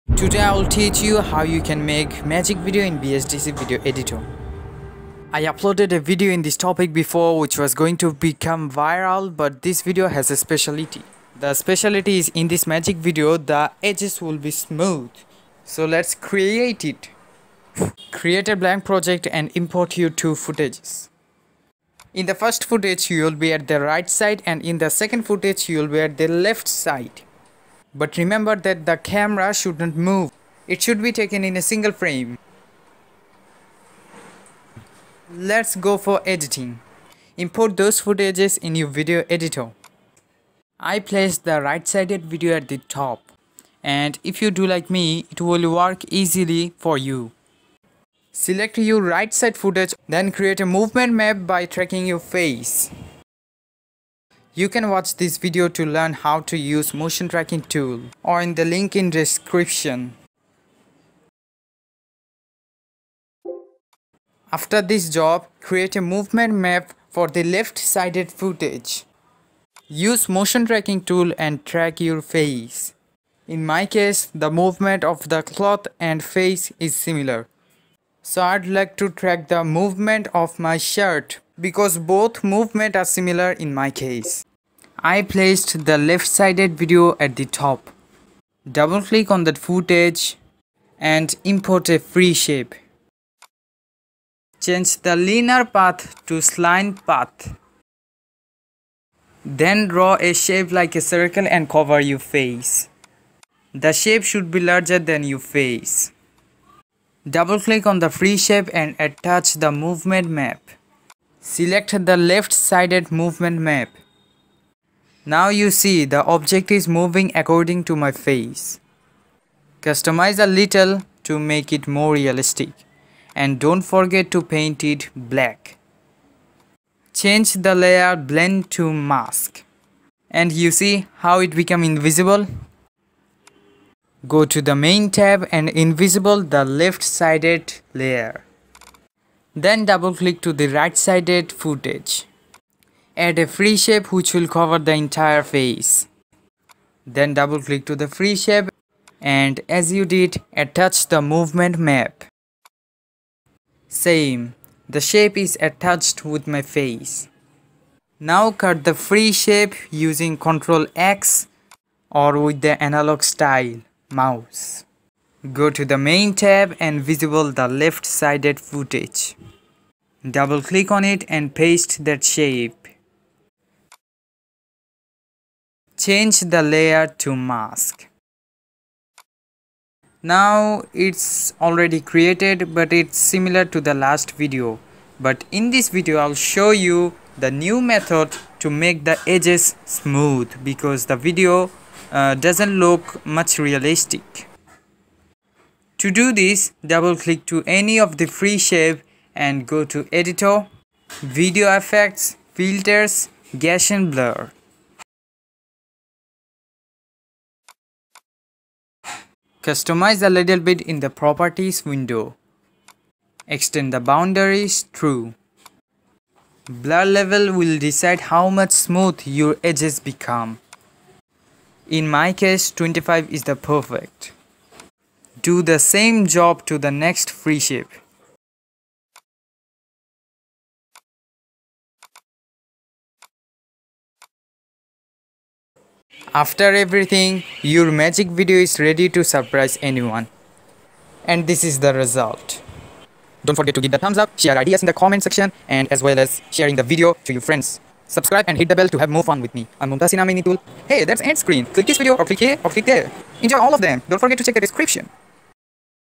How to make a Magic Video by VSDC Free Video Editor | 2021 Advanced VSDC Tutorial
Content made by,
Mumtahsin Amin Nitul

This a tutorial video of VSDC Free Video Editor. VSDC is free and amazing video editor. Some of the tricks in this video require pro version of vsdc, but you can continue by VSDC Free version [To export, you must need pro version].

If you have any question, please comment. Hold a like if this video helpful to you. Thank you very much. Try to create some amazing videos.